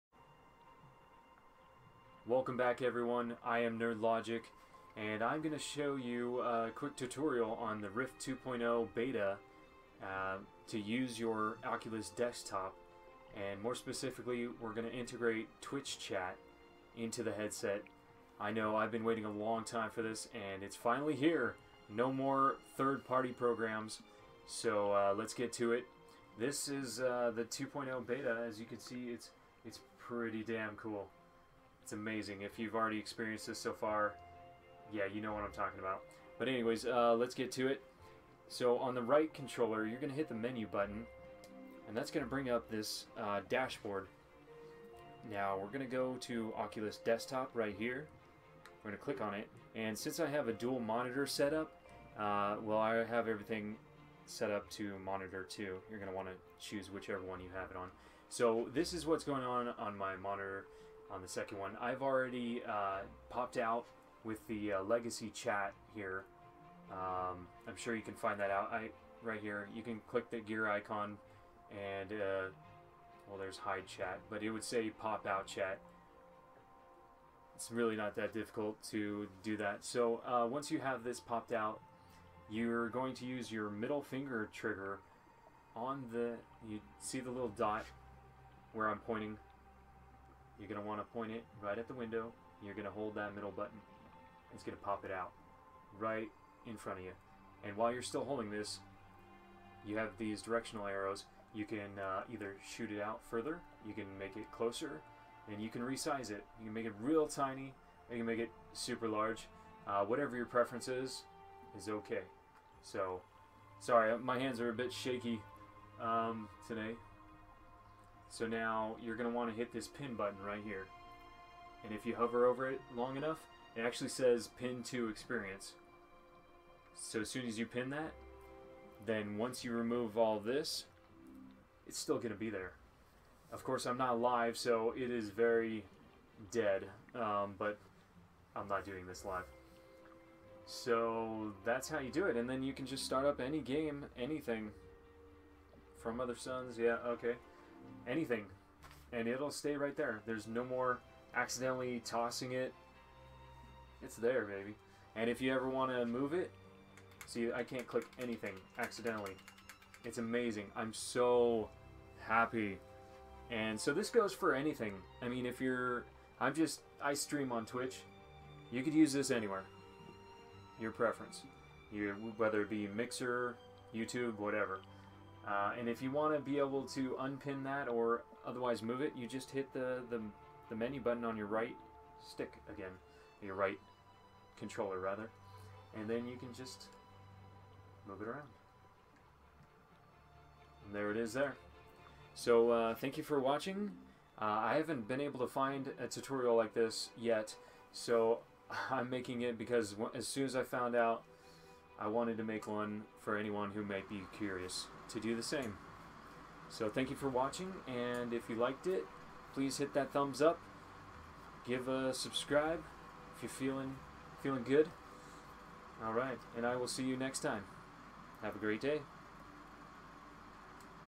Oculus Rift 2.0 Beta Twitch Chat Integration
This is a quick tutorial on how to get Twitch chat into the Oculus Rift headset, using the new Oculus Rift 2.0 Beta version of the software.